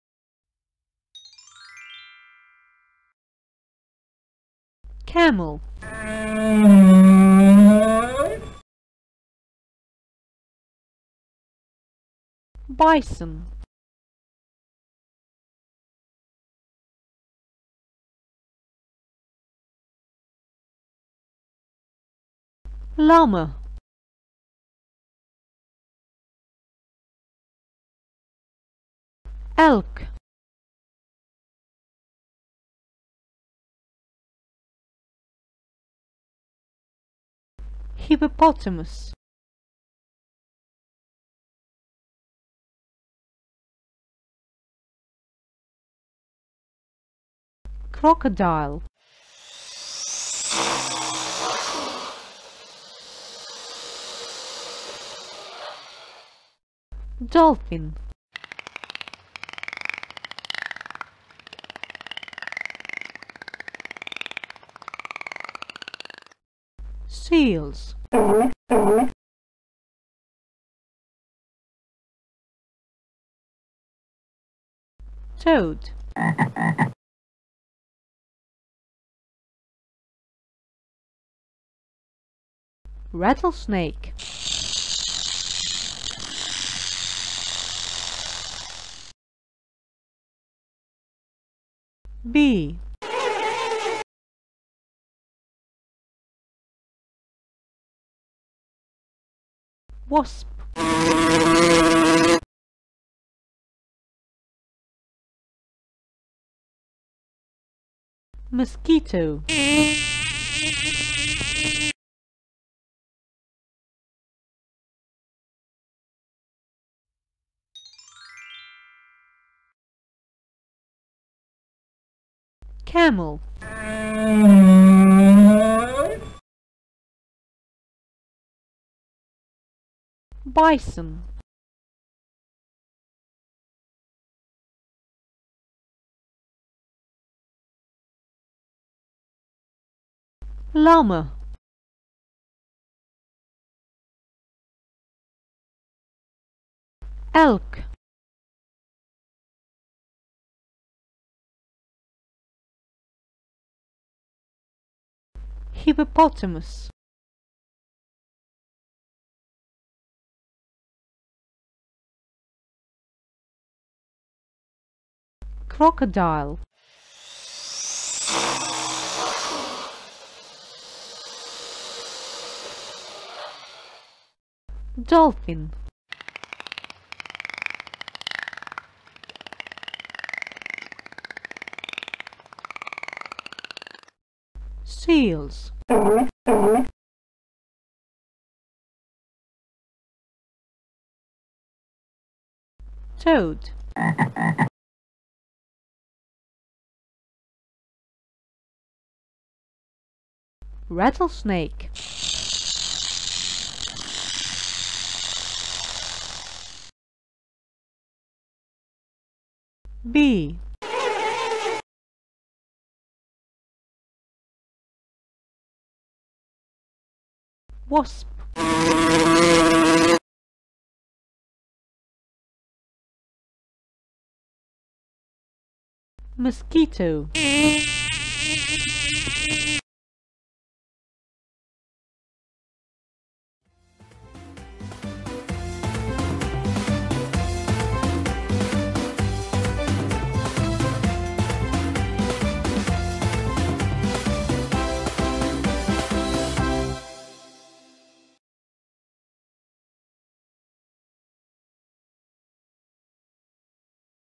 Wild Animals 2 - Vocabulary Flashcards
English vocabulary video lesson with talking flashcards. Wild Animals Part 2. Use this video lesson to learn English words for wild animals.